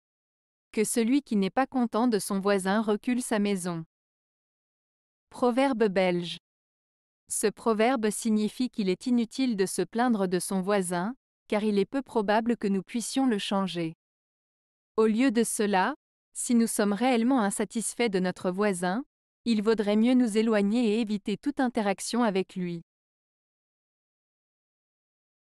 Que celui qui n'est pas content de son voisin recule sa maison. - Proverbe Belge
Ce proverbe signifie qu'il est inutile de se plaindre de son voisin, car il est peu probable que nous puissions le changer. Au lieu de cela, si nous sommes réellement insatisfaits de notre voisin, il vaudrait mieux nous éloigner et éviter toute interaction avec lui.